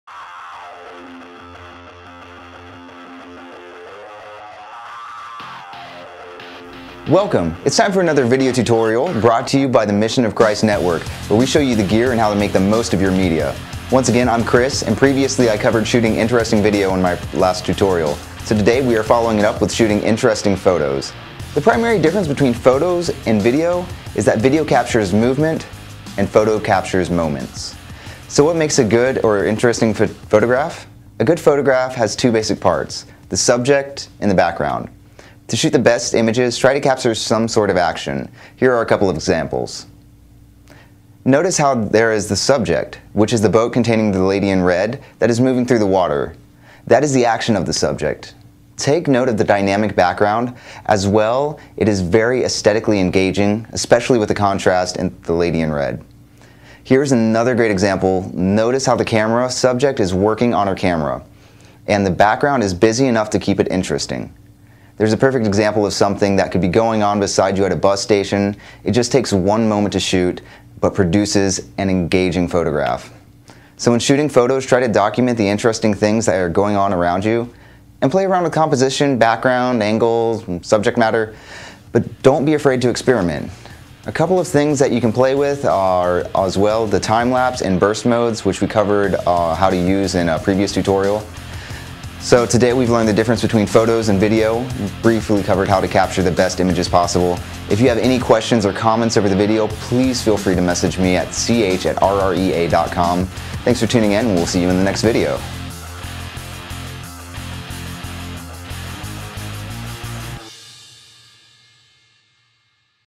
Media Tutorial: Shooting Interesting Photos
Mission of Christ Network shows how to take more interesting photos using your GoPro.